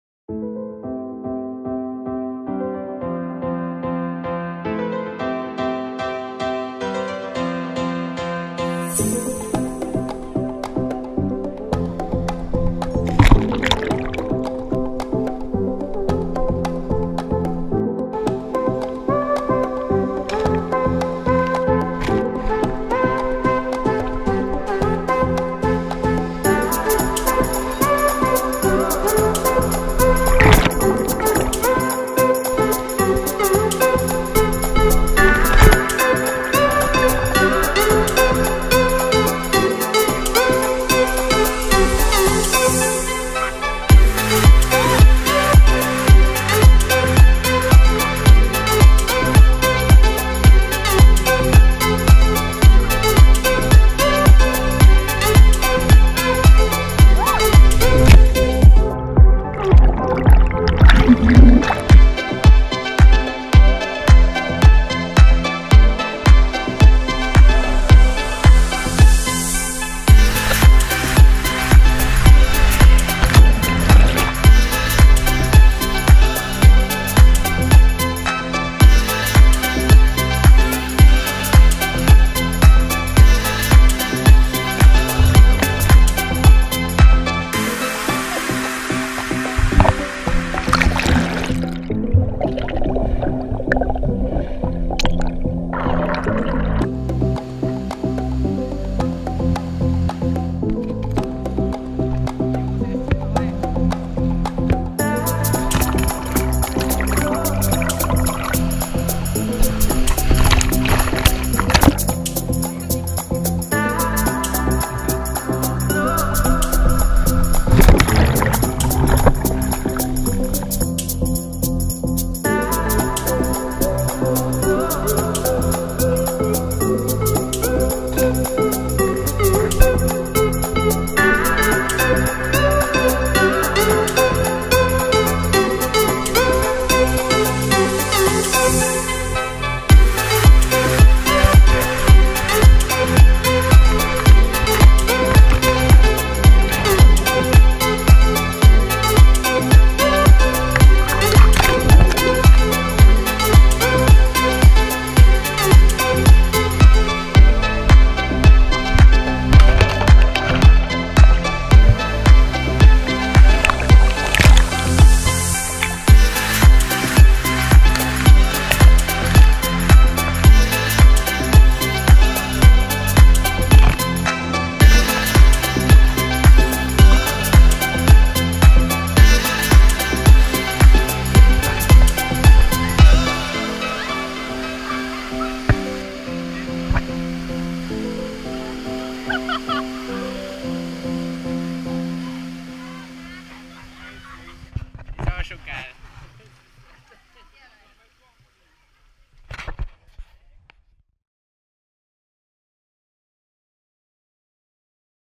Extreme CANYONING in Corsika 2017
Eines der weiteren Highlights waren die zwei Canyons/Schluchten auf Korsika, die weltweit einzigartig sind.
Ich freue mich außerdem über jedes Kommentar und über jede Bewertung.

► Filmed with: / Gefilmt mit:

~Nikon D 3300 Objektiv
~GoPro Hero 4 Black
~Mein Dome für Unter- und Überwasservideos und Fotos
~DJI Phantom 3 Advanced

Nilssports2k17